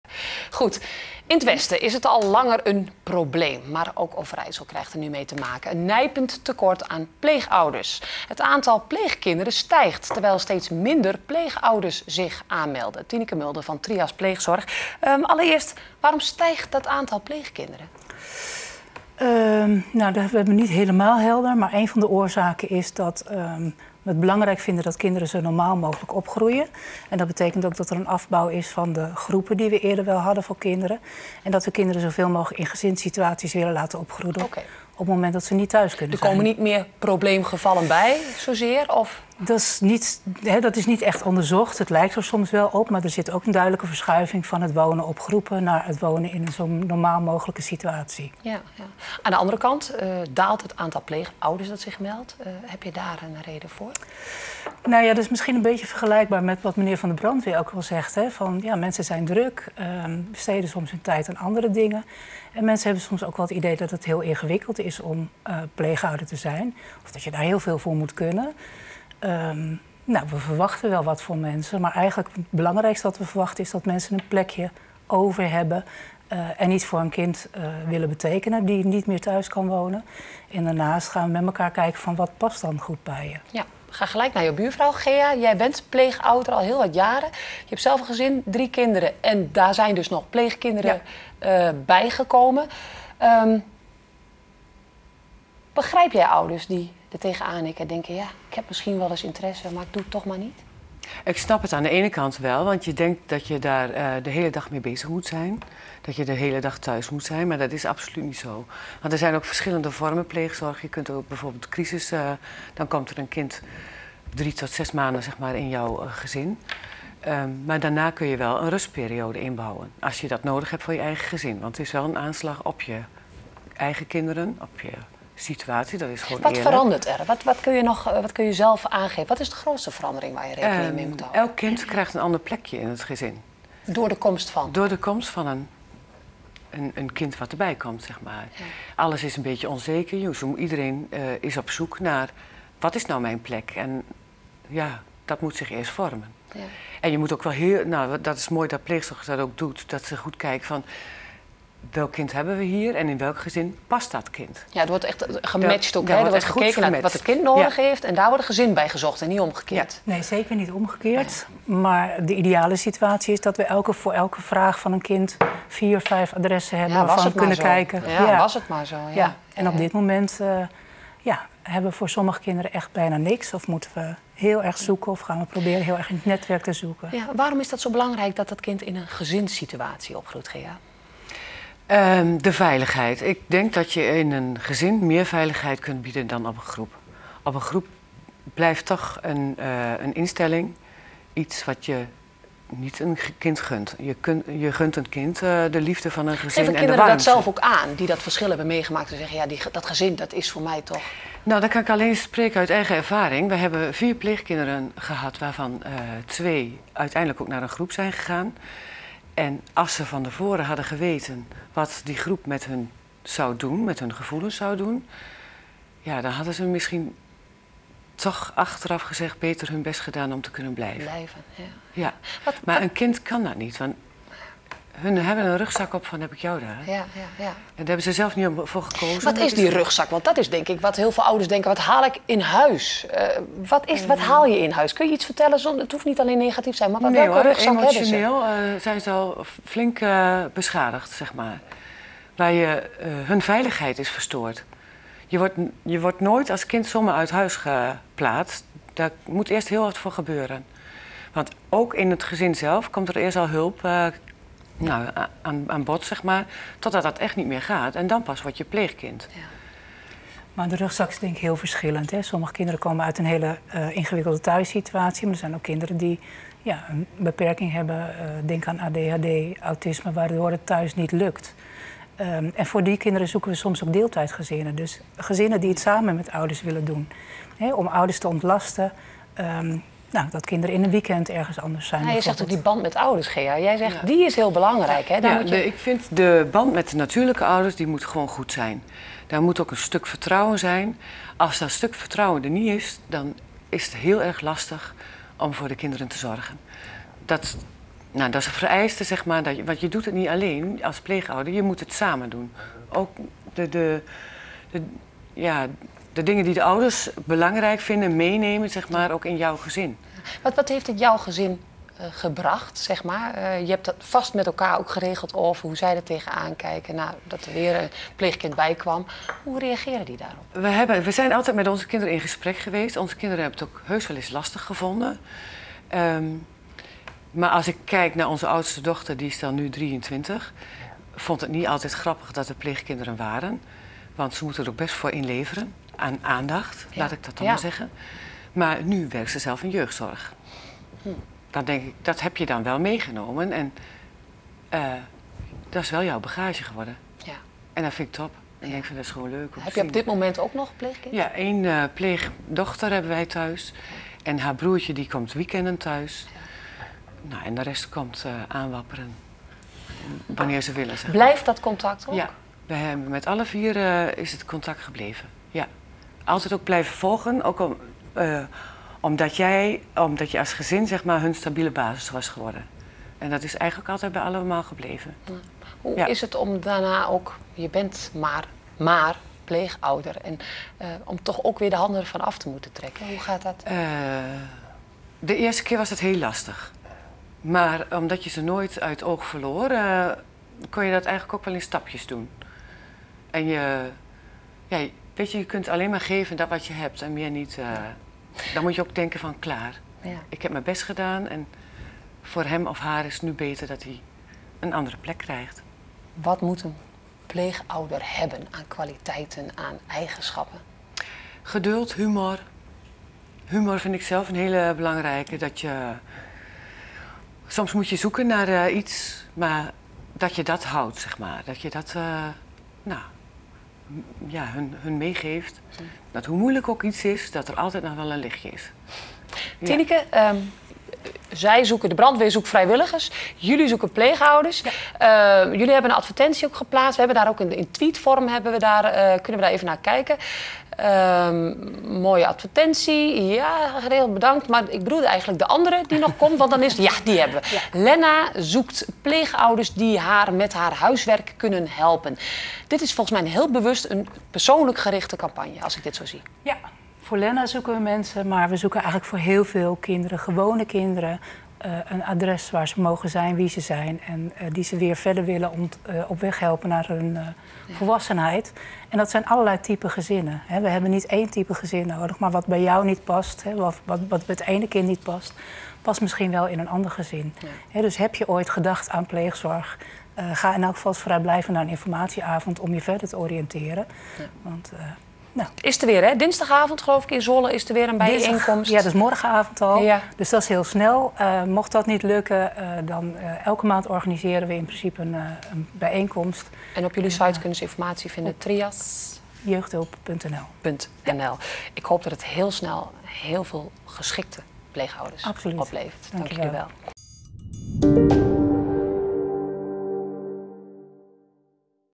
Gezocht! Pleegouders in Overijssel
Het wordt voor de Trias Groep steeds moeilijker om genoeg pleegouders te vinden in Zwolle en omgeving, "In het westen is al een tekort aan pleegouders en dat ontstaat nu ook in Overijssel", aldus Tineke Mulder van Trias.